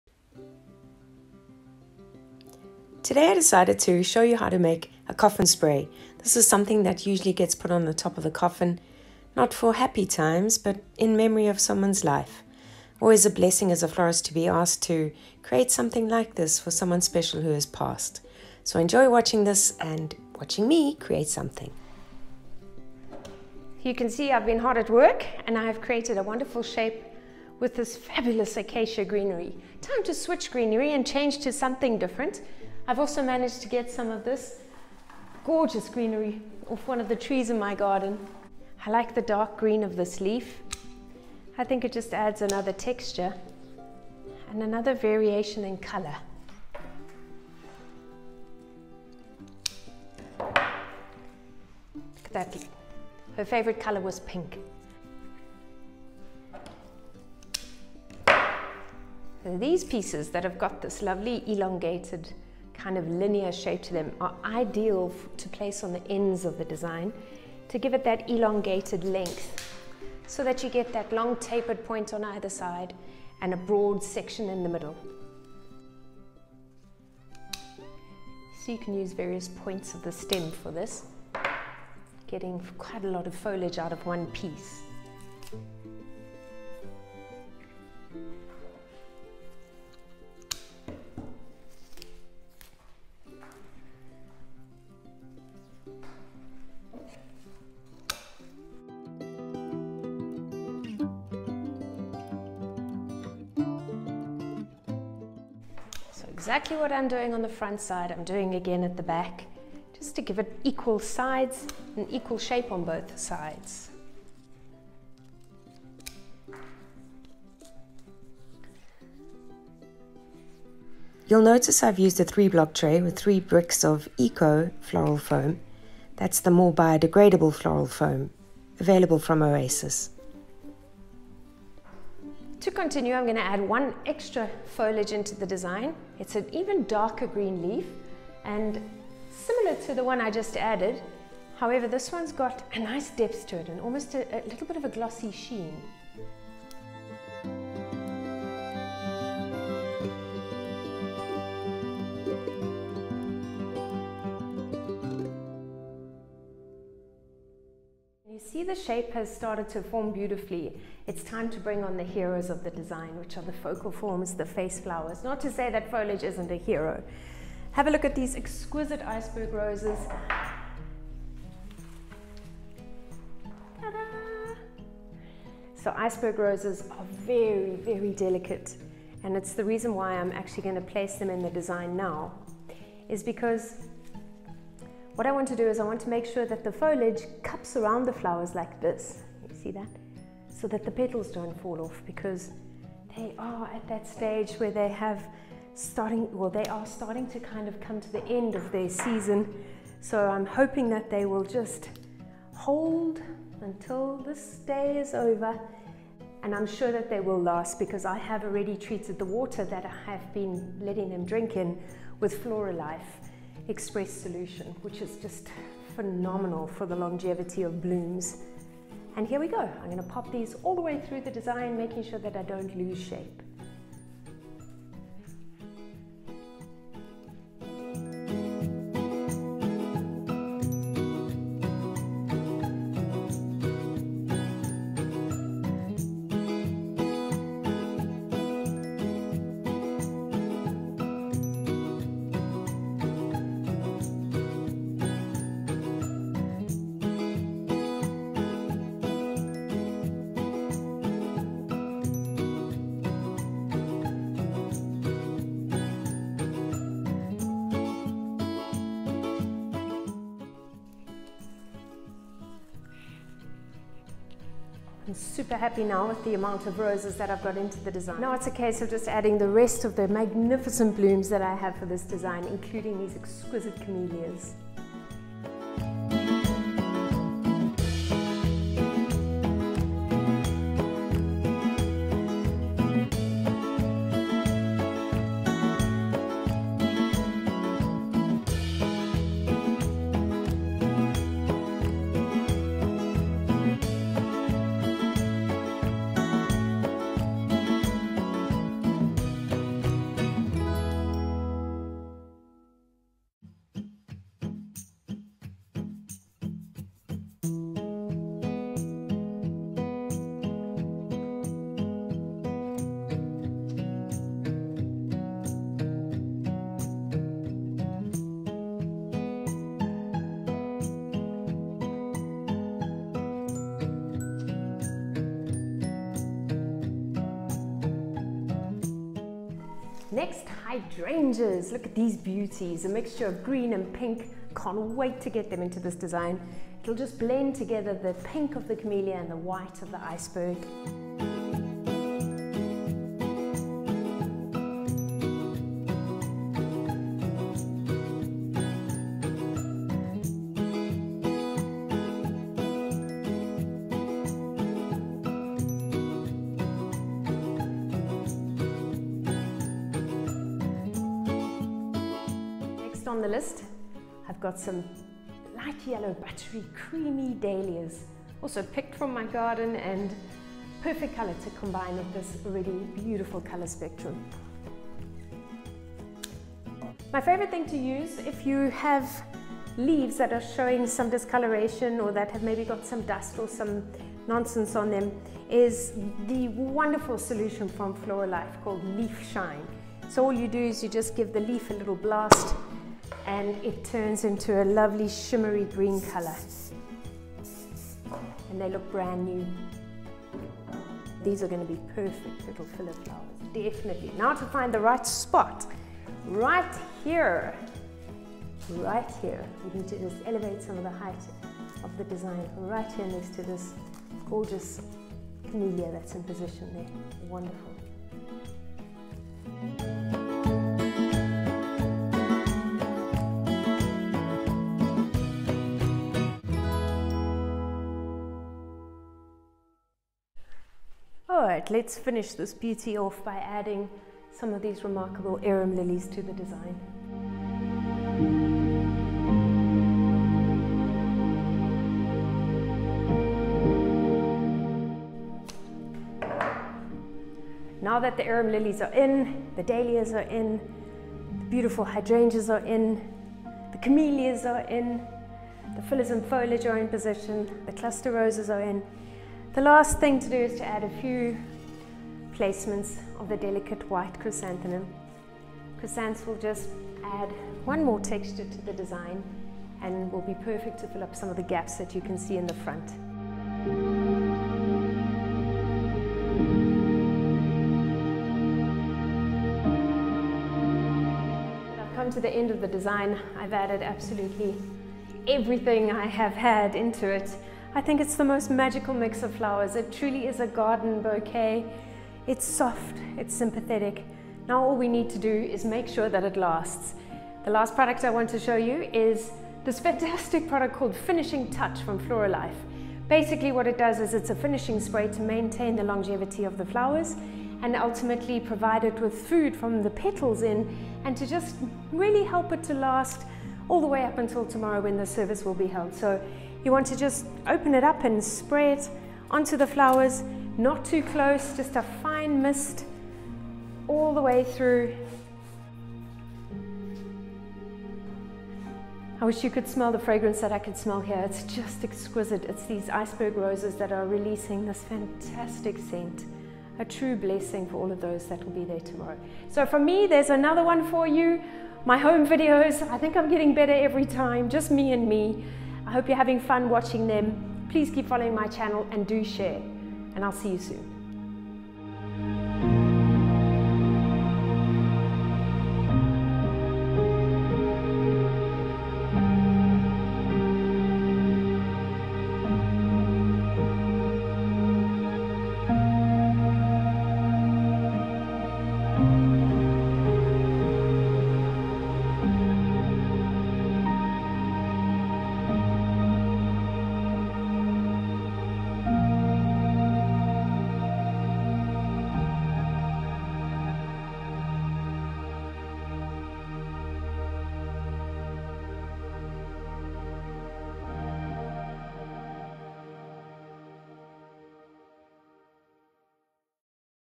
A coffin spray made from the garden
A funeral for a someone who loved her garden inspires the selection of flowers for this coffin spray that I made.